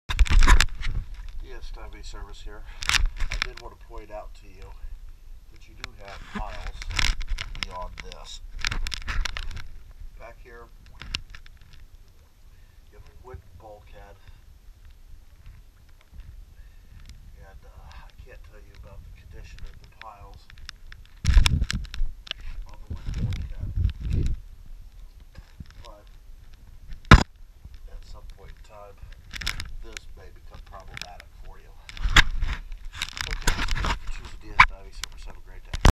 Baisdon Rd pile survey
This video shows the bulkhead behind tour falling concrete seawall.
We didn't crawl back there to get a good look but you can see it pretty well from the angle I got.
This will be a problem in the not so distant  future!!